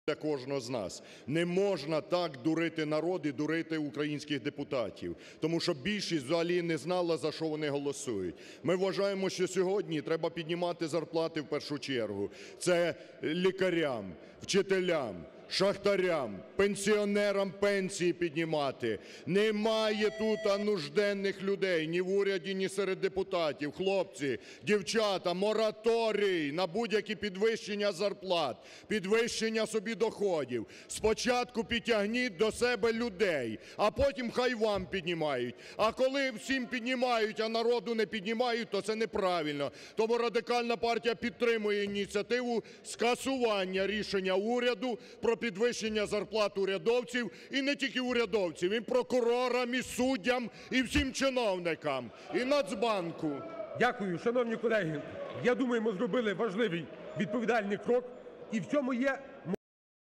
Ляшко вимагає мораторію на підвищення зарплат чиновникам
Народний депутат України, лідер опозиційної фракції Радикальної партії Олег Ляшко заявив про те, що піднімати зарплати депутатам неприпустимо.
"Немає тут у Парламенті нужденних людей. Повинен бути мораторій на підвищення зарплат чиновникам, поки люди не живуть нормально", - наголосив Ляшко.